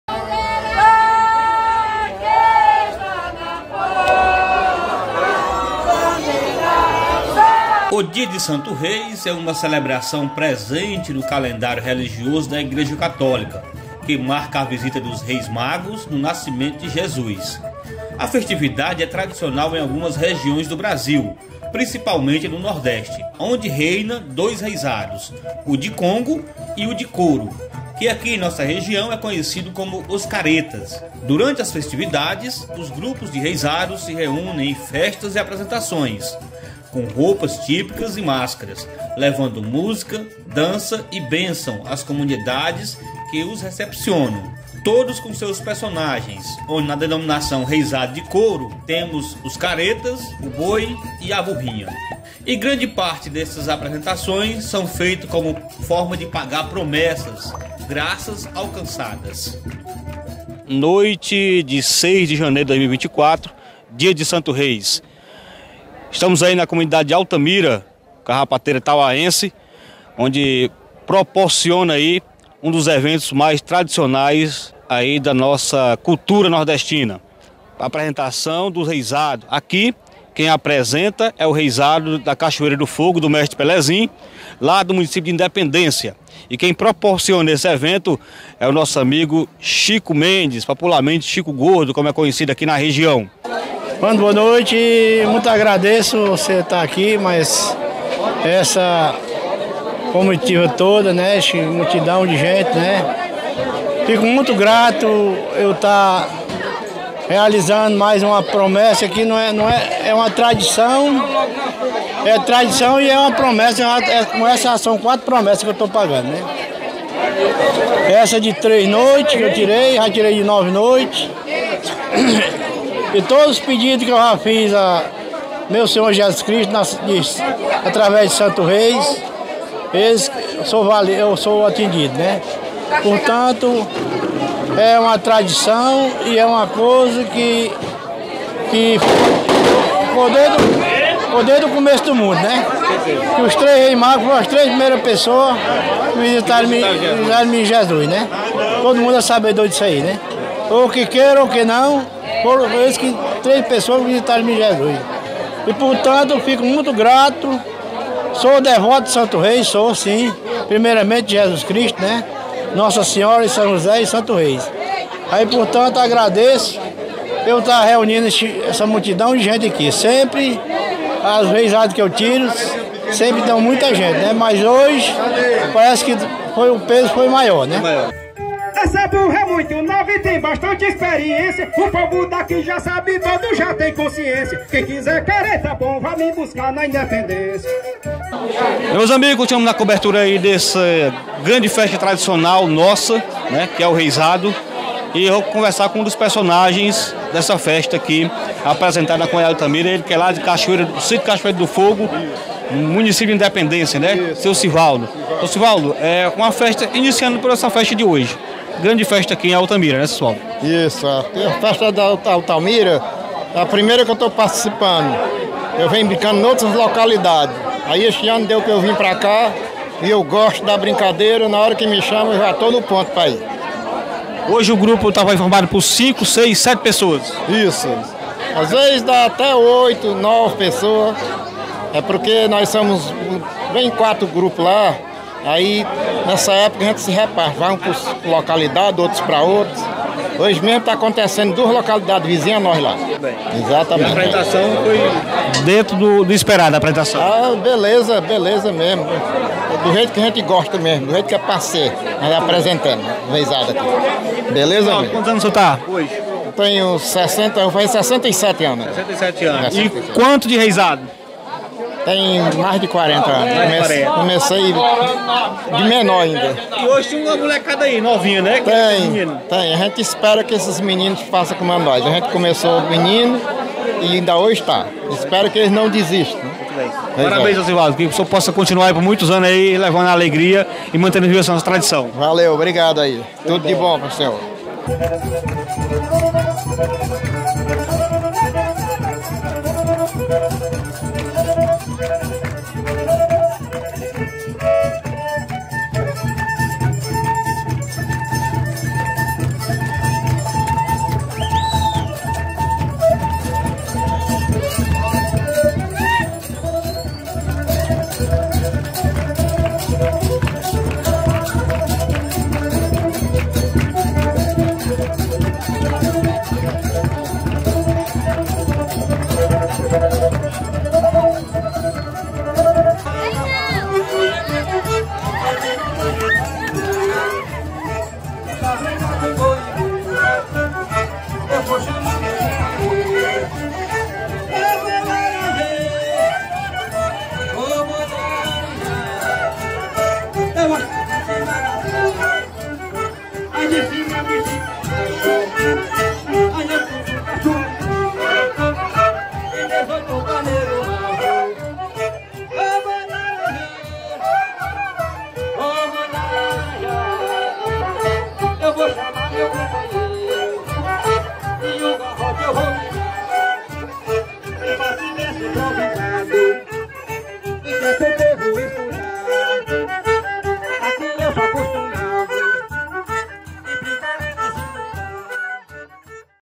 Festa de Santo Reis se mantem viva com seus reisados no inteiro do Ceará
Dia de Santo Reis foi de festa na comunidade de Altamira, município de Tauá.  Onde um grande público se fez presente na noite deste sábado (06), acompanhando  a tradicional Festa promovida por Chico Mendes e família.

O grupo originário de Cachoeira do Fogo , município  de Independência-CE